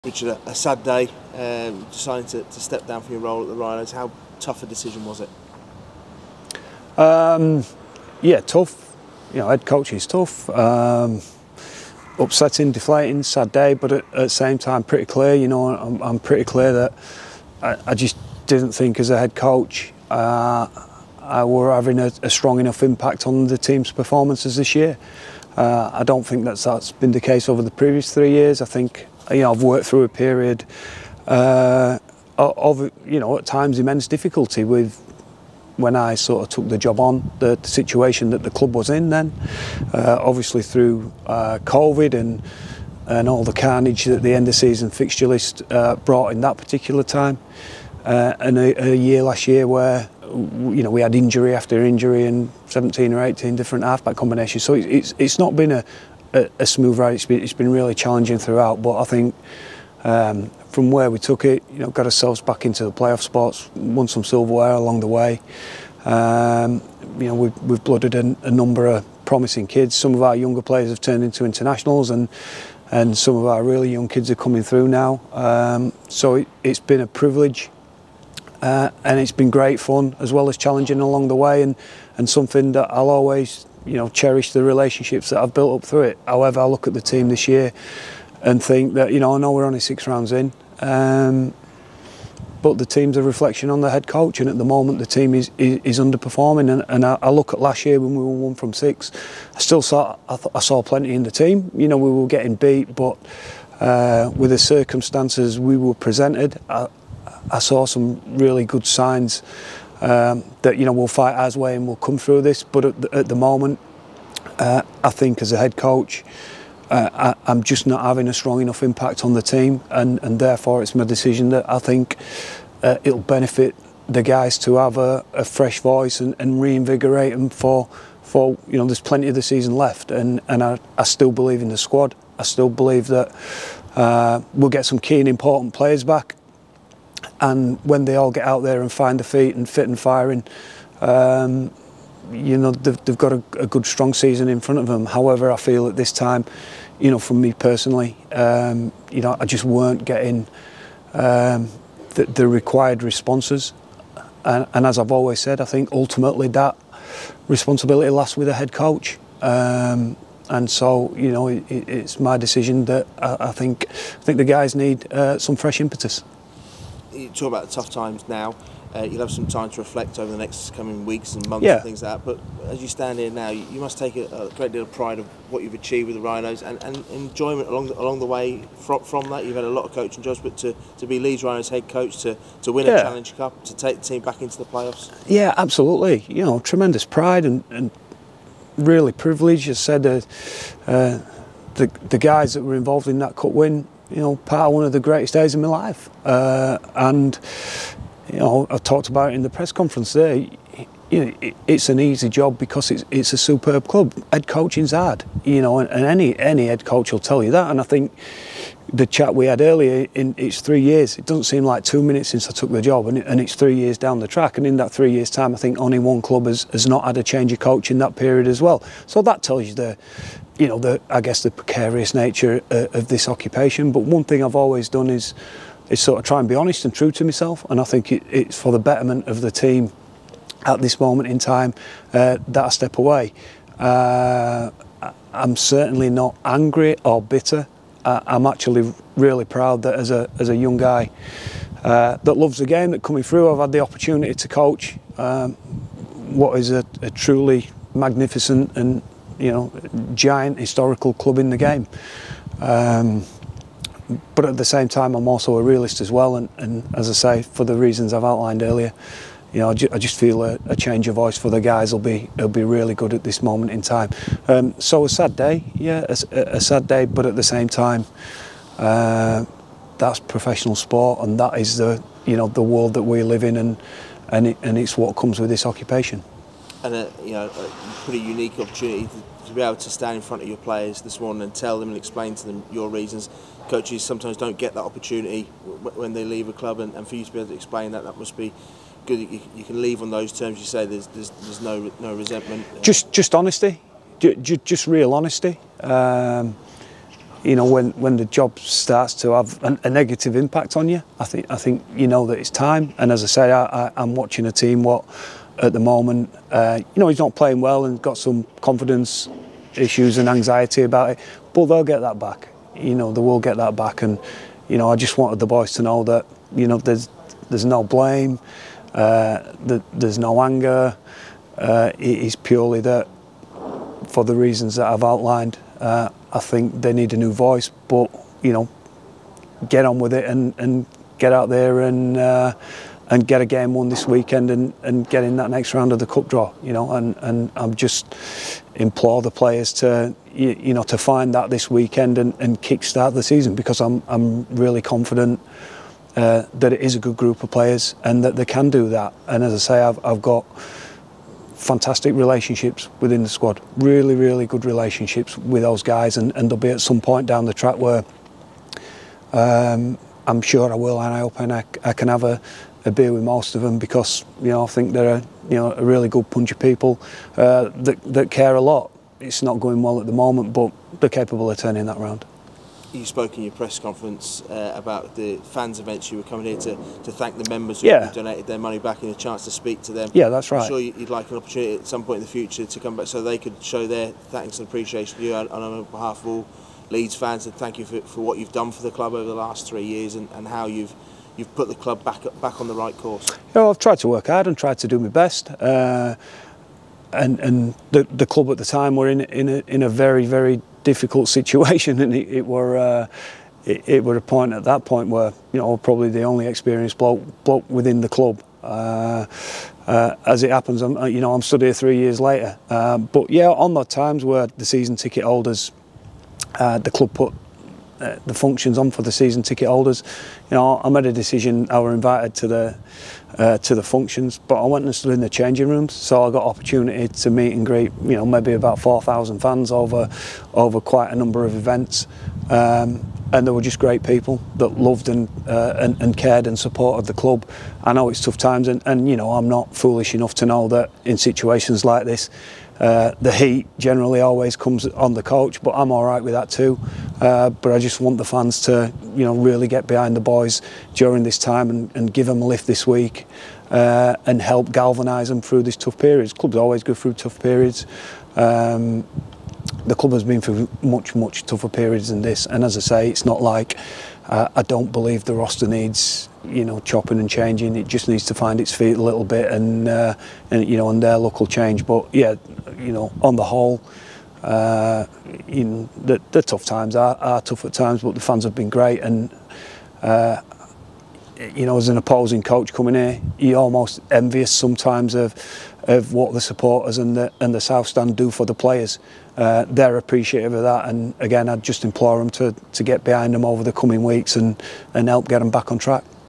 Agar proud of achievements with Rhinos
Departing Leeds Rhinos Head Coach Richard Agar said whilst he was deflated and upset to be leaving the club, he does so proud that he has been able to help the club get back in the right direction with an outstanding crop of young players emerging through the ranks.

Agar decided to step down as Head Coach and has thanked the club's supporters for their backing and urged them to get behind the team this week ahead of a crucial Challenge Cup clash with Castleford Tigers at Headingley on Saturday.